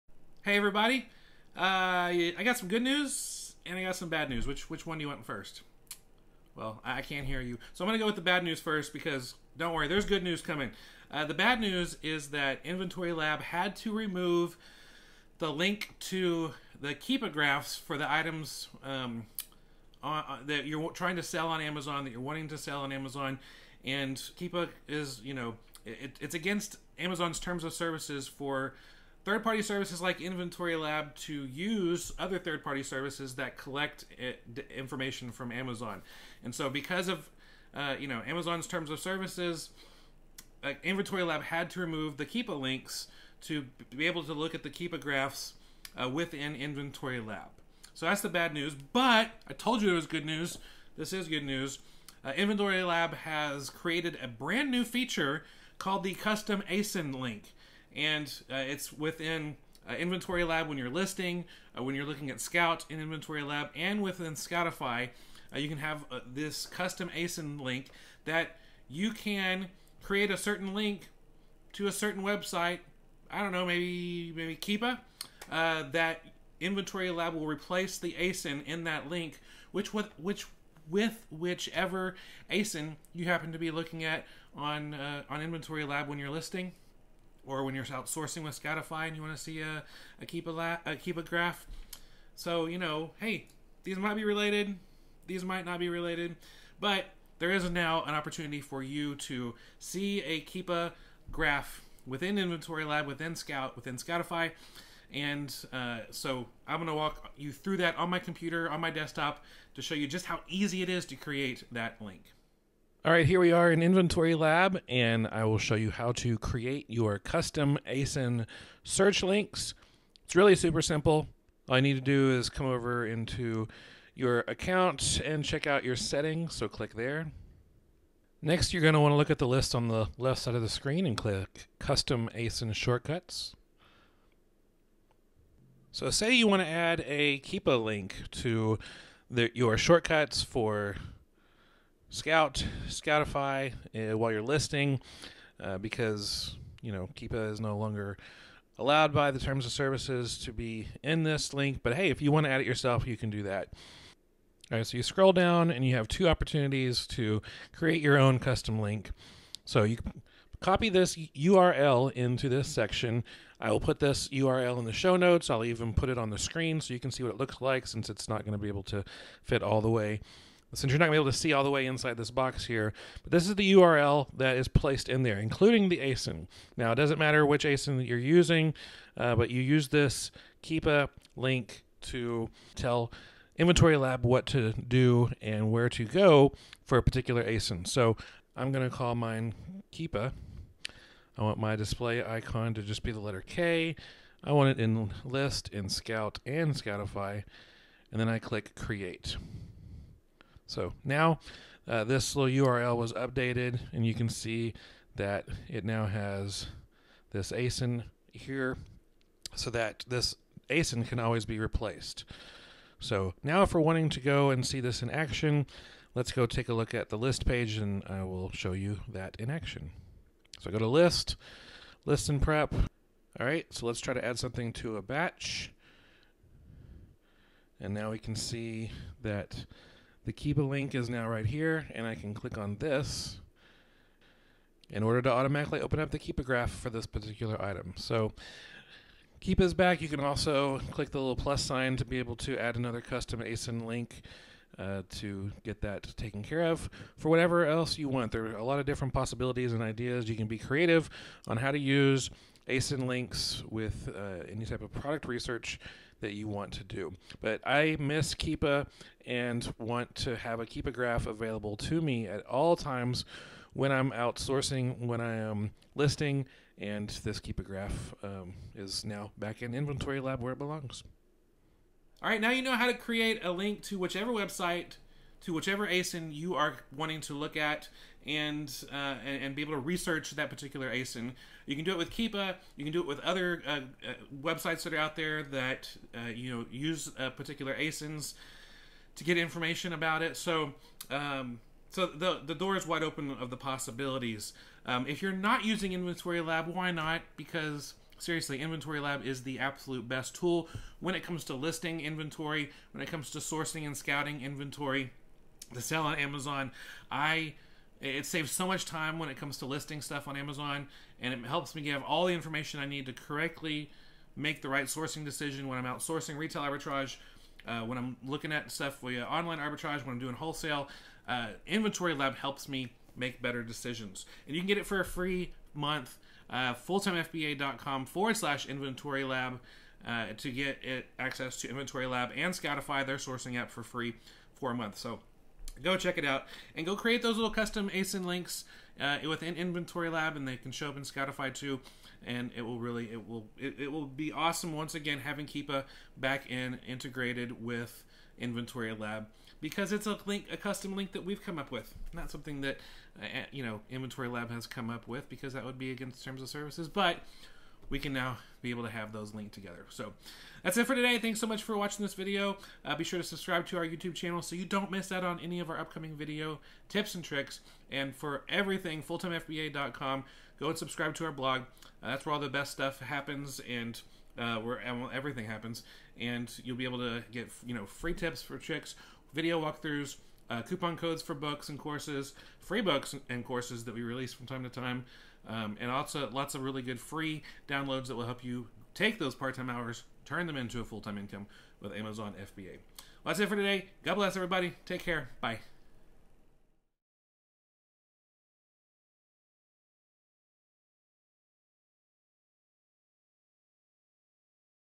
Inventory Lab Scoutify ASIN Link Tool - How to Add Keepa Links (and more) Back Into InventoryLab
In this Amazon FBA training video, Stephen walks through how to use the ASIN link tool to use Keepa with InventoryLab and the Scoutify app. Don't worry about Amazon removing the Keepa link from Scoutify!

Links in the video:

The text to use with the Custom ASIN Link Tool set-up

Hope this video is helpful! Leave your questions and comments below!